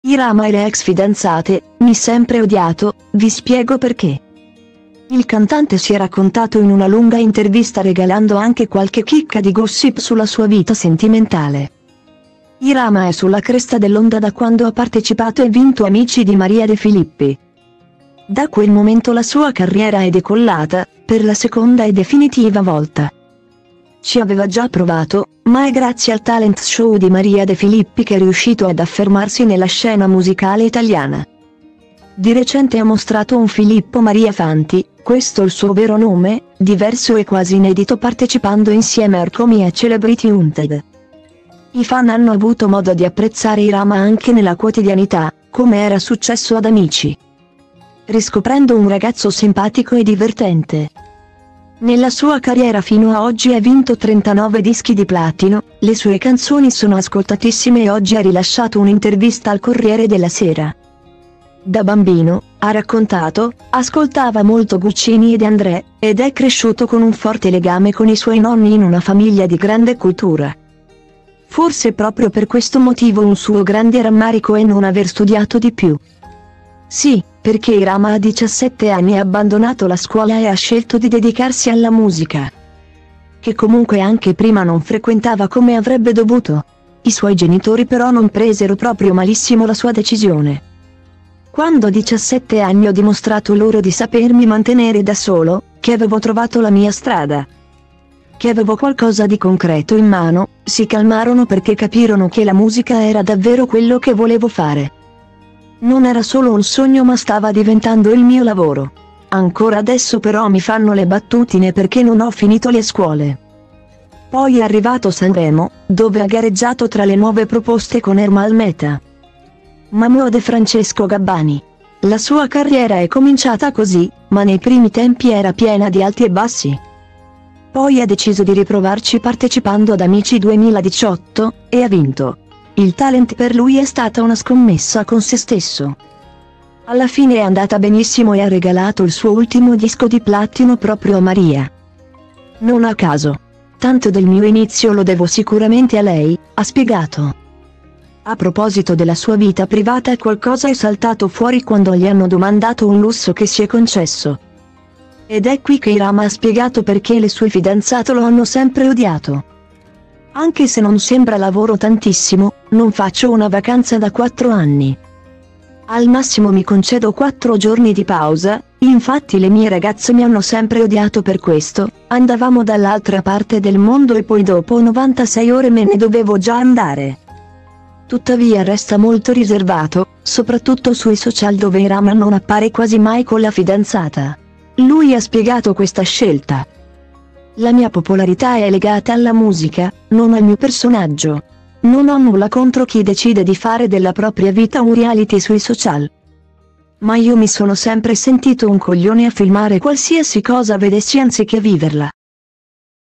Irama e le ex fidanzate: "Mi sempre odiato, vi spiego perché"
Il cantante si è raccontato in una lunga intervista regalando anche qualche chicca di gossip sulla sua vita sentimentaleIrama è sulla cresta dell’onda da quando ha partecipato e vinto Amici di Maria De Filippi.Da quel momento la sua carriera è decollata, per la seconda e definitiva volta.